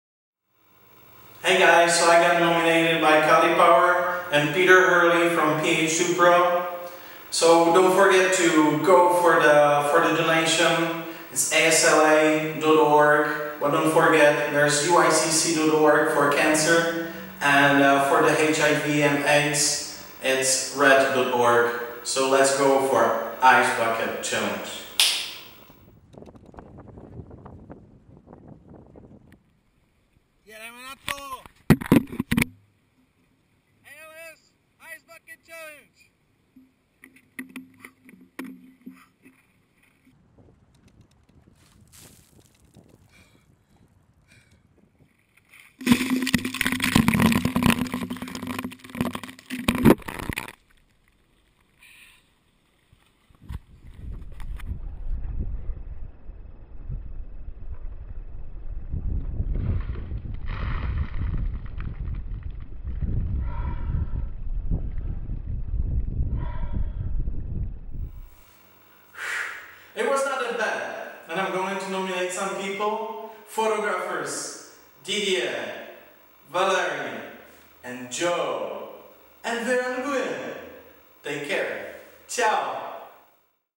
Robert Hradil
I accepted the challenge from Cali Power and Peter Hurley. After donation I made the Ice Bucket at Geneva, hoping the Jet will be showering on me, but they turned it off due to the wind.

Do not forget to spread the word about all other causes like cancer, HIV/Aid or NCDs.